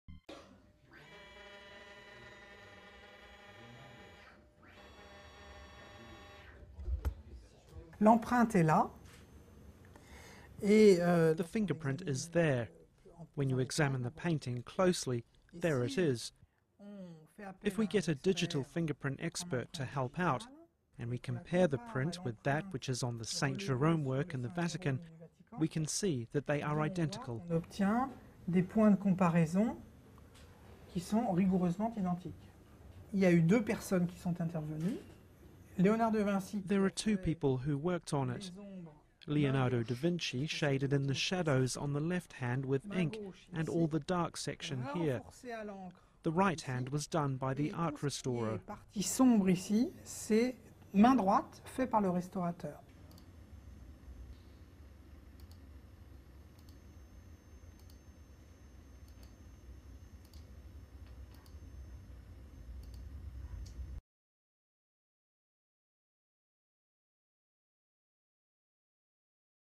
AFP Moderne science uncovered DaVinci
The number one press agency, reveal to the word an incredible discovery.A new Leonardo Da Vinci paintig has been dicovered using a new multispectrale camera. (Lumiere Technology)
Carbon 14, Fingerprint and many technical and physical element prove it.